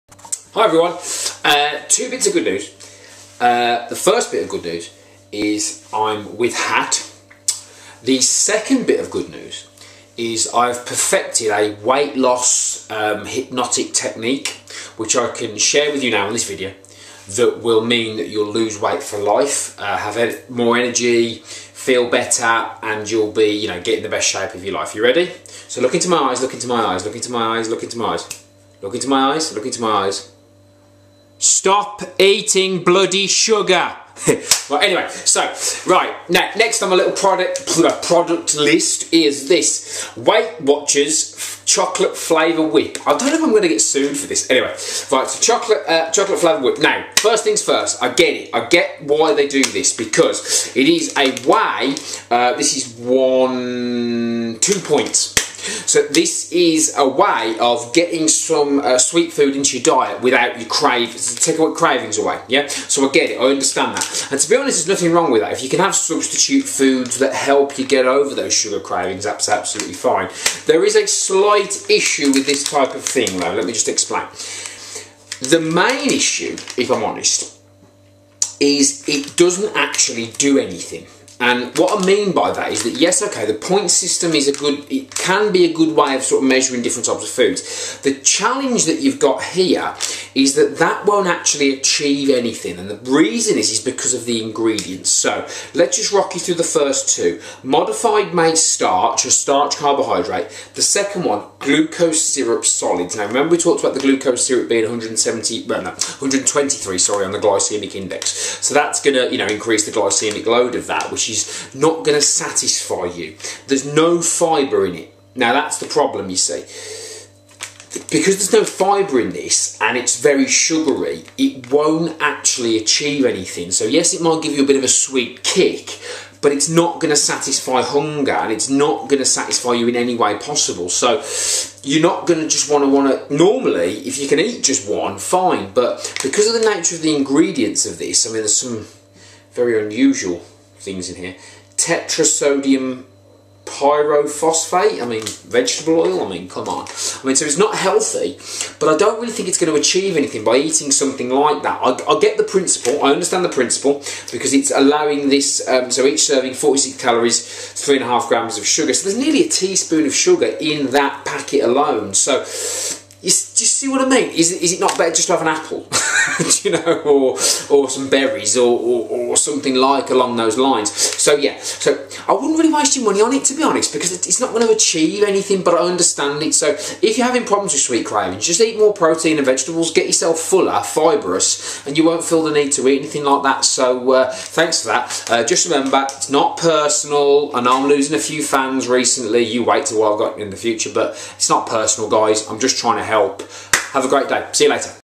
Some Cool Weight Loss Hypnosis & More Sugar Revelations
Check This Weight Loss Hypnosis And More Fun And Games With Sugar & Weight Loss Products